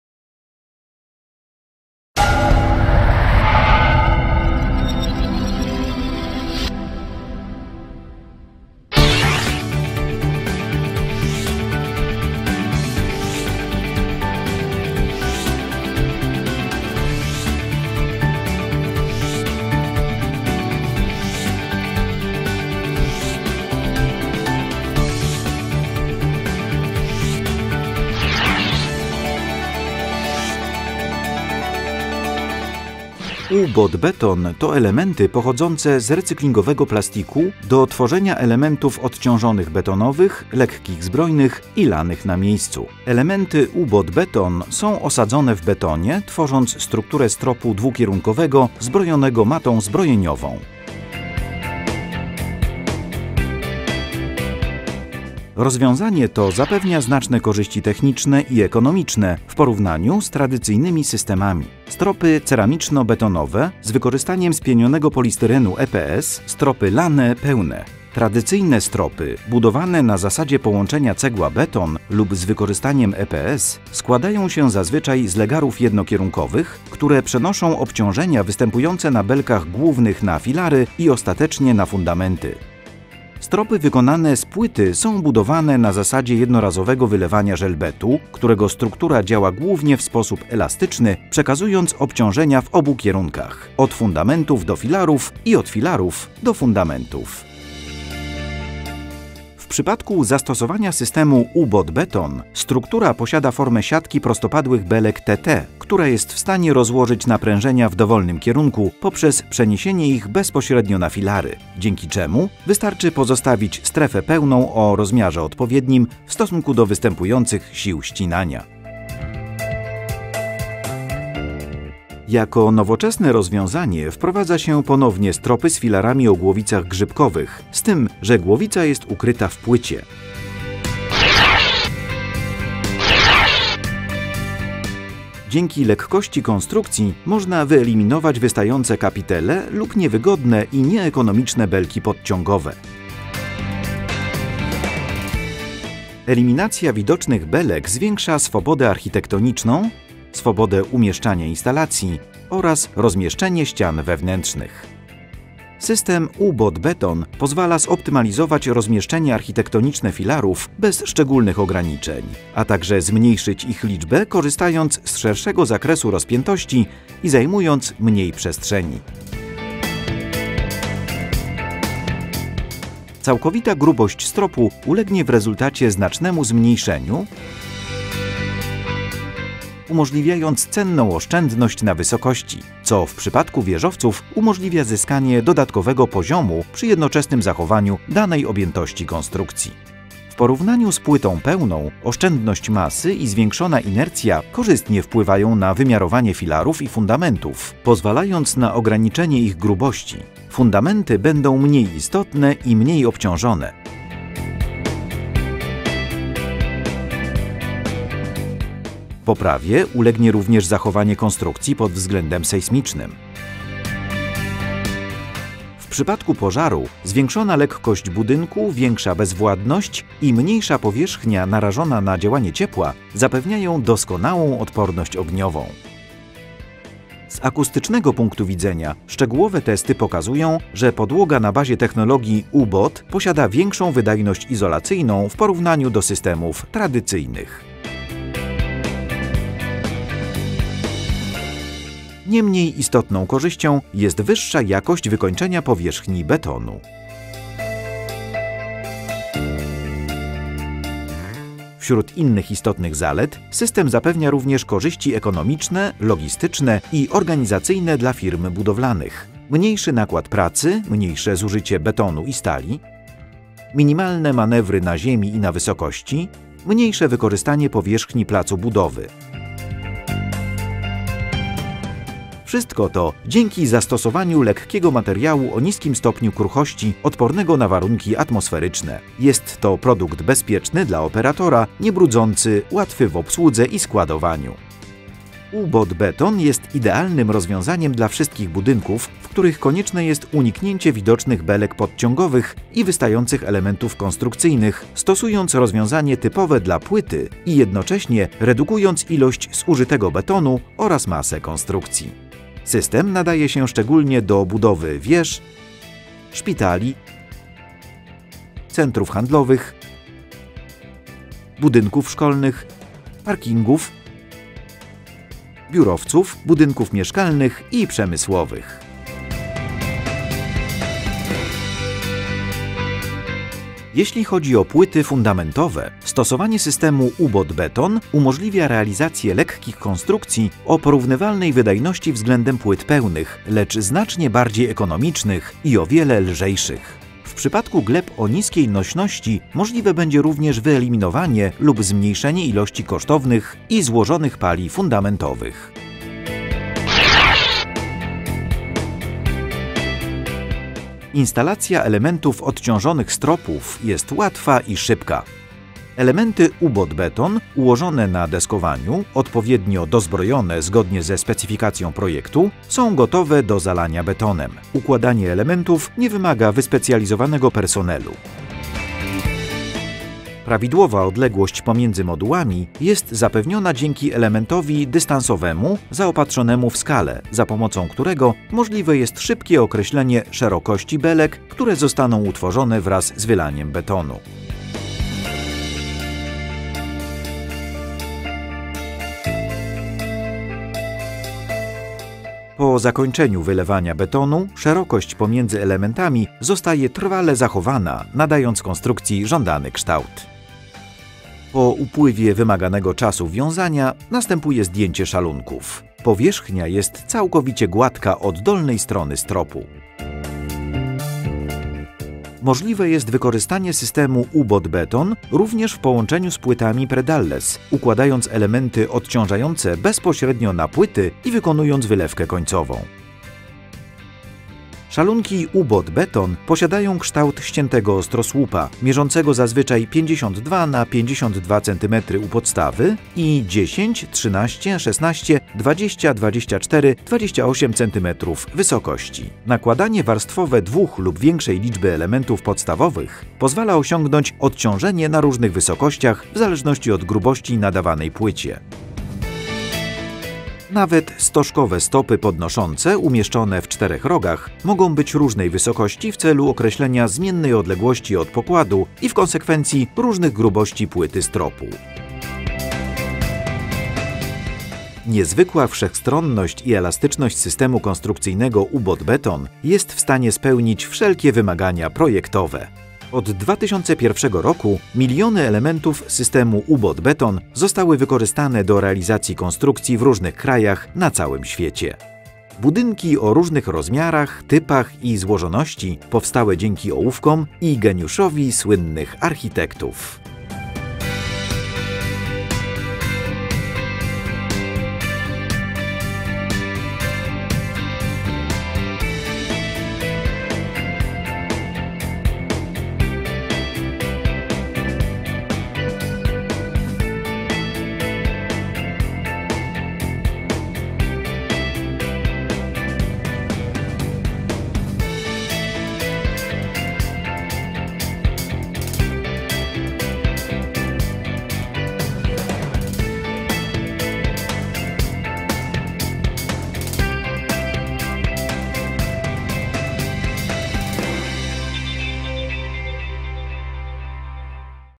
U-BOOT® BETON - stropy o konstrukcji dwukierunkowej bez wypełnienia
System U-Boot® Beton jest wykorzystywany we wszystkich budynkach, które wymagają zastosowania płyty strukturalnej wraz ze zmniejszoną ilością betonu, a tym samym w celu uzyskania lekkiej konstrukcji.
System U-Boot® Beton stanowi świetne rozwiązanie dla wykonywania stropów od dużych rozpiętościach i bardzo dobrej nośności: doskonale sprawdza się przy realizowaniu konstrukcji, które wymagają zastosowania otwartych przestrzeni, np. biura, budynki komercyjne i przemysłowe, a także obiekty publiczne, cywilne i mieszkalne.

Filary mogą zostać rozmieszczenie z większą nieregularnością, gdyż nie zachodzi potrzeba wykonywania belek. W przypadku placów budowy z utrudnionym dostępem lub w przypadku odbywania się dodatkowych prac konserwacyjnych, system U-Boot® Beton można wykorzystać do wykonania poziomych struktur konstrukcji bez potrzeby stosowania urządzeń służących do podnoszenia lub przenoszenia elementów budowlanych, gdyż moduły zajmują mało miejsca oraz są lekkie i poręczne.
Stosując system U-Boot® Beton można również wykonać płyty fundamentowe o większej grubości i przy mniejszej ilości betonu.